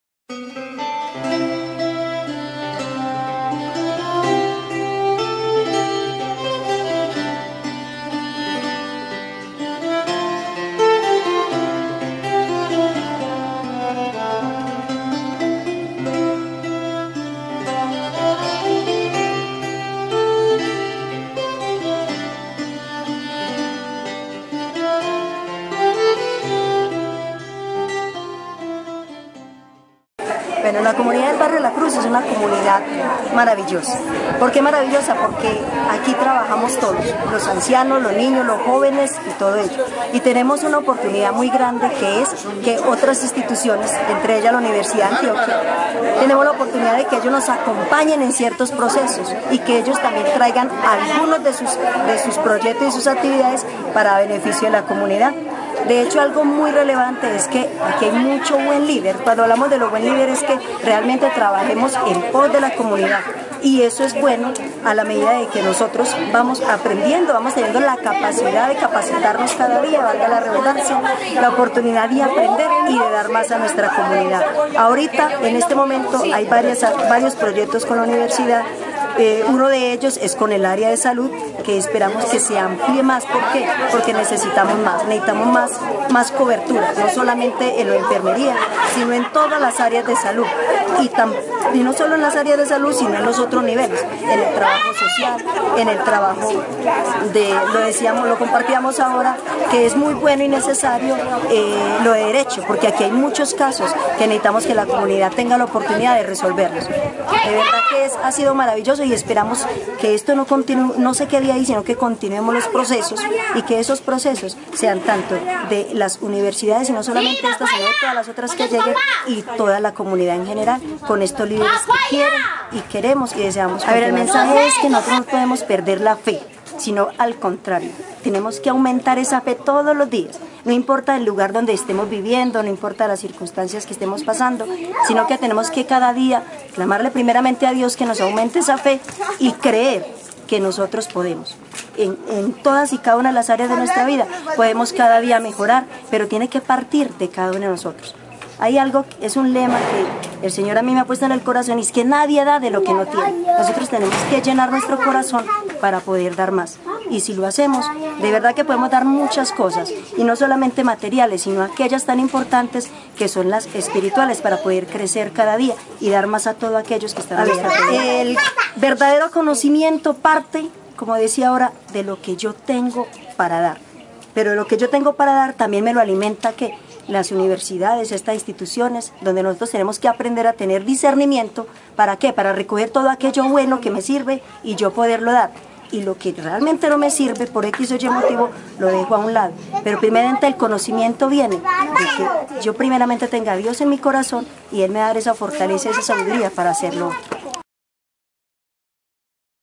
ORSALC - Entrevista Sra. Luz Giraldo - Lider Comunal del barrio La Cruz.wmv
ORSALC - Entrevista Sra. Luz Giraldo - Lider Comunal del barrio La Cruz
Responsabilidad Social y Ciudadanias.